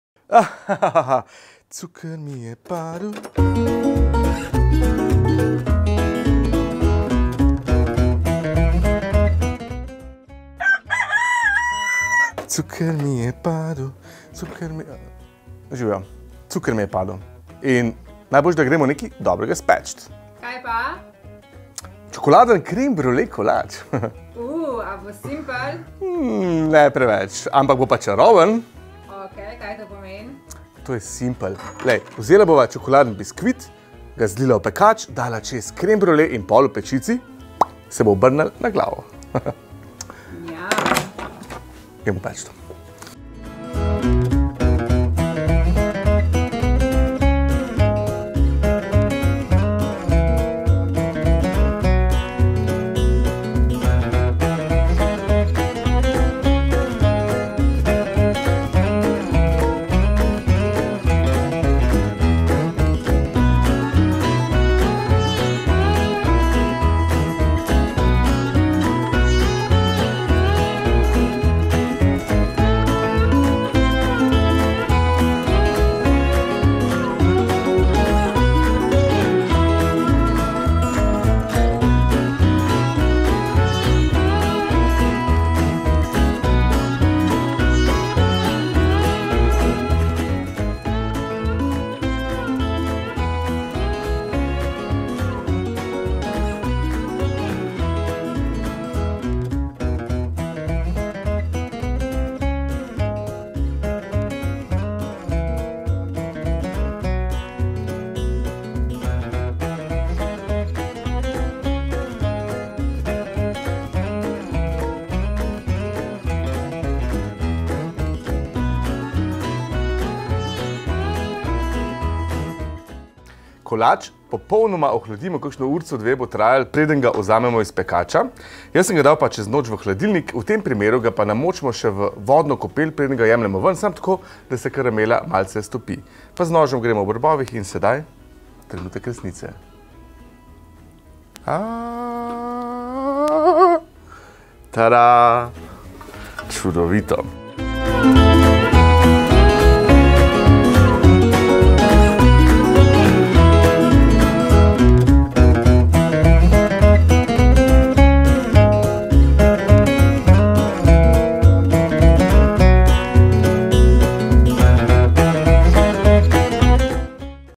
Creme brulee kolač 😍 [DOMAČA KUHNA]
Creme brulee kolač je božansko okusna sladica, ki jo spečemo v enem pekaču a ima kar tri različne plasti. Ponudi nam bogat, sočen čokoladni kolač, ki ga prekriva gladek creme brulee in karamelni preliv. Kaj naj rečem? V enem grižljaju smo deležni koščka nebes 😍

V pekač najprej vlijemo karamelo, nato biskvit in povrh še kremo. Med peko pa krema steče na dno in rezultat je slasten obrnjen kolač s kremo na vrhu ❤

Njami.